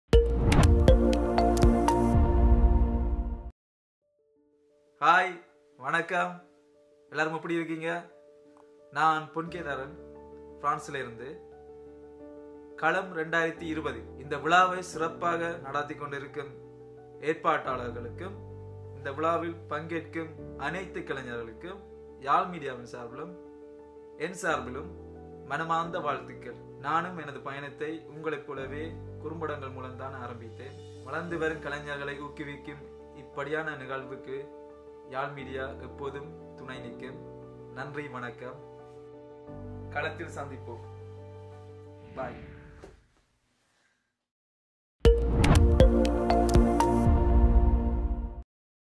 Pon Ketharan’s message for Kalam 2020 Awards in India
En somme, je félicite cette initiative et je leur ai souligné que moi même j’ai débuté cette aventure par des courts métrages . Et que je soutiendrai ce genre d’événements avec Yaal Media 🎬